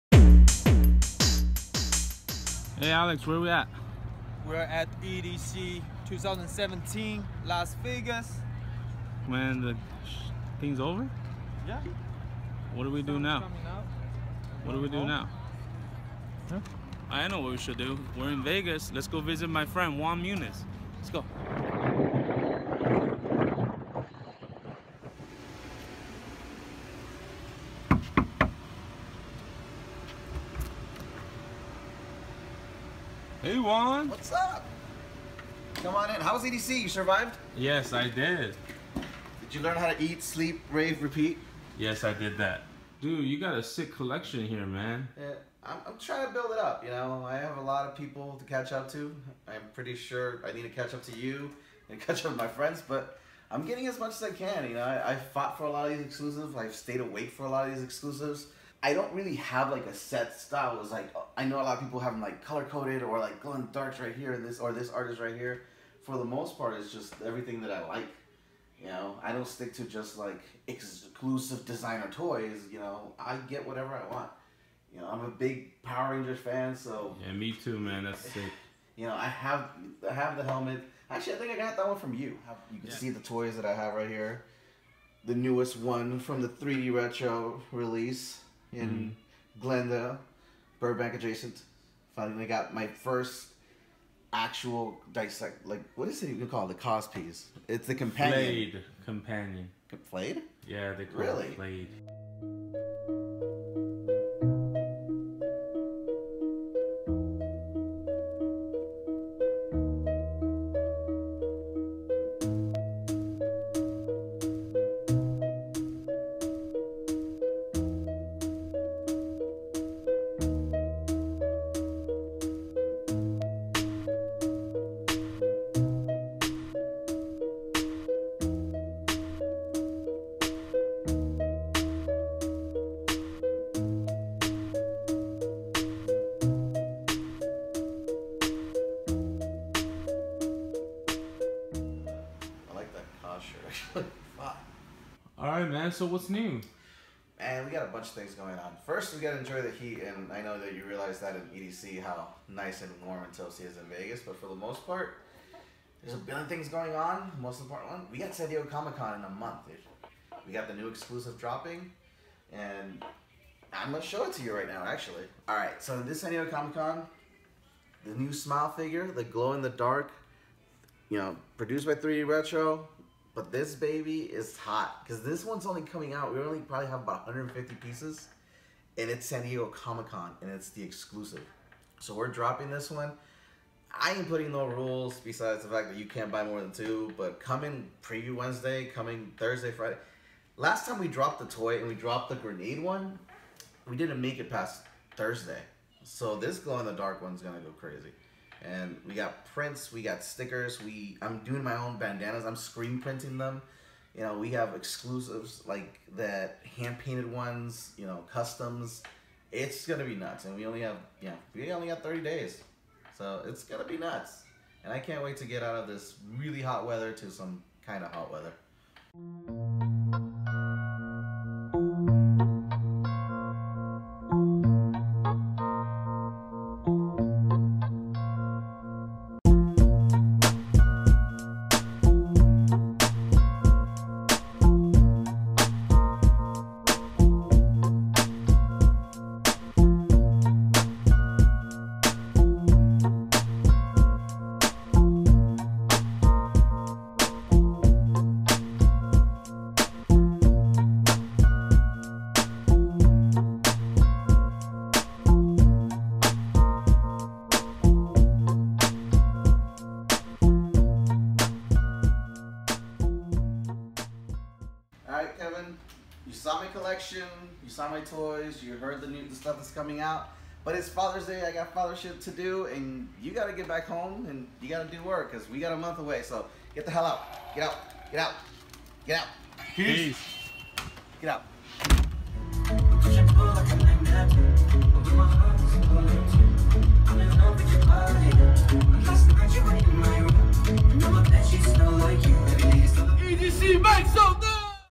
What's New Episode 6: DoWork with Juan Muniz
Visited the hardest working artist in Las Vegas, Juan Muniz.  Checked out his personal toy collection and talked about what's New for San Diego Comic Con 2017.

Follow DeKevin on Instagram: @dekorner @_life_of_kai
Follow Juan Muniz on Instagram: @ninobuni @jmunizart
Follow Leo on Instagram: @earwaxx